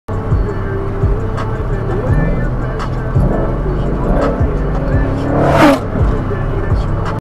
Fly by in my 2016 Mustang GT
2016 Mustang GT with Stainless Works long tube headers with cats and Borla sport cat back exhaust. Taken from the dash cam in my sons 2014 Mustang GT.